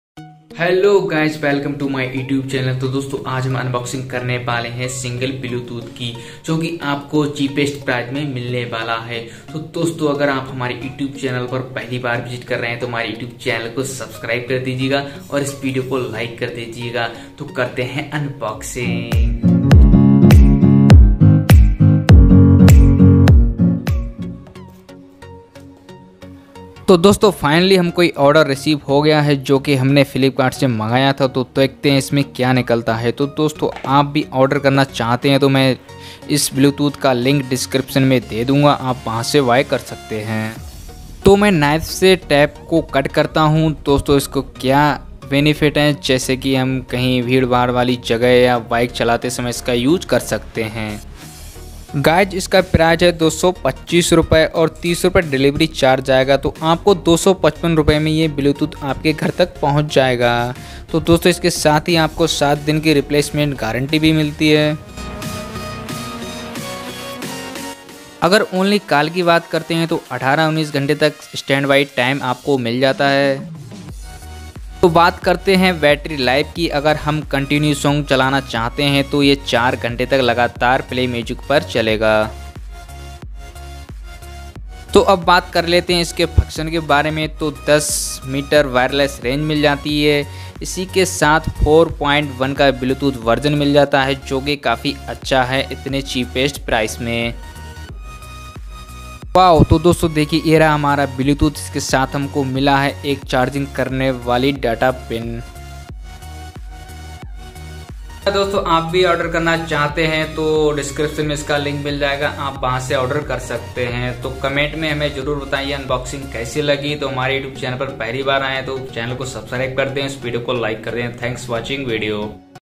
Single Wireless Bluetooth device for mobile / only 225 में / bluetooth Unboxing and review
Buy bluetooth Link
SkyKey TIG-i single-75 Bluetooth Headset